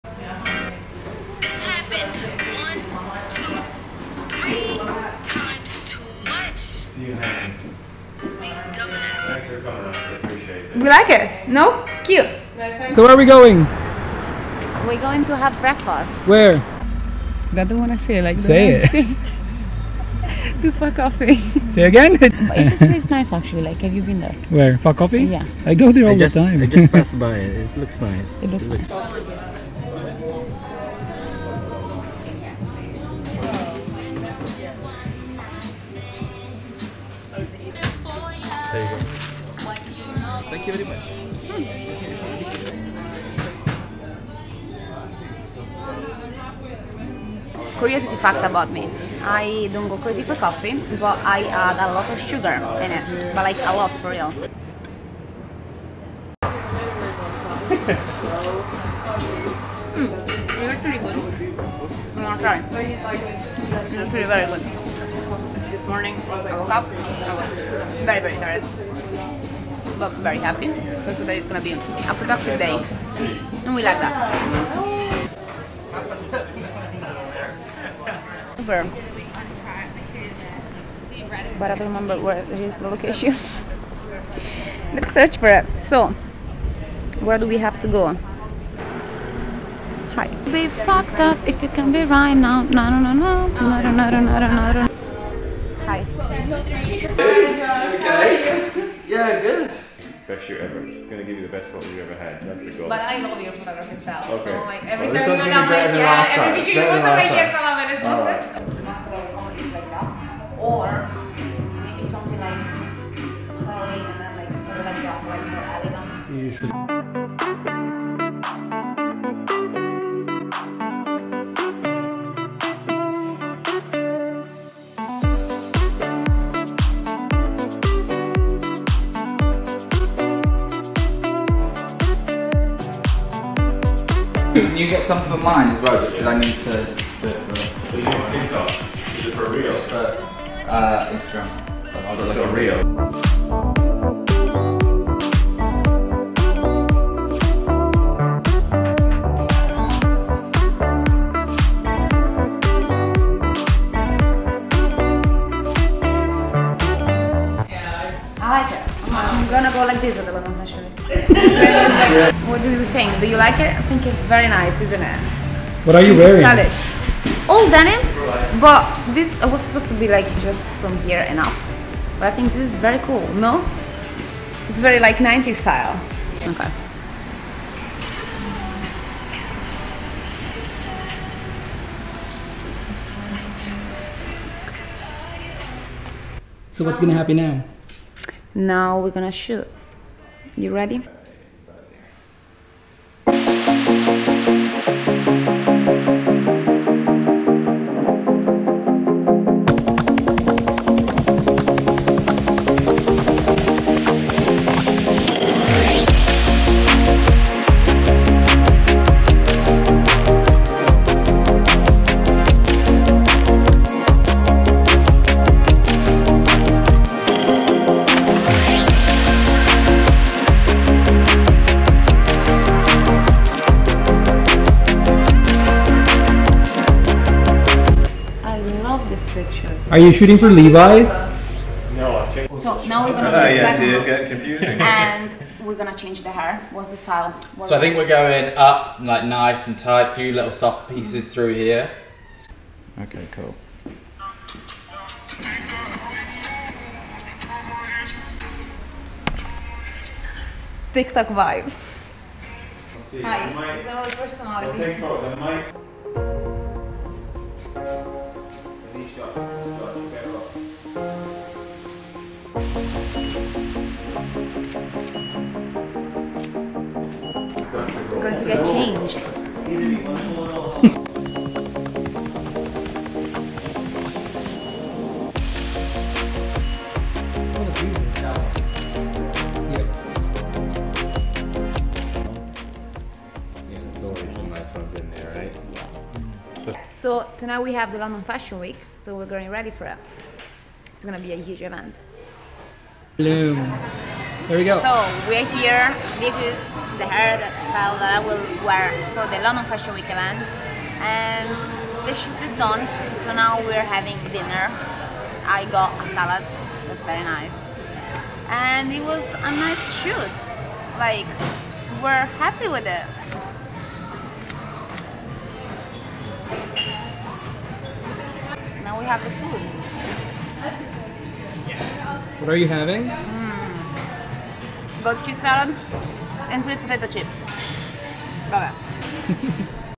Day in the Life of a Model | Diary of Marygrace Tropeano // vlog
I bring you on set with me. Hope you like it!

Production: Studio Midori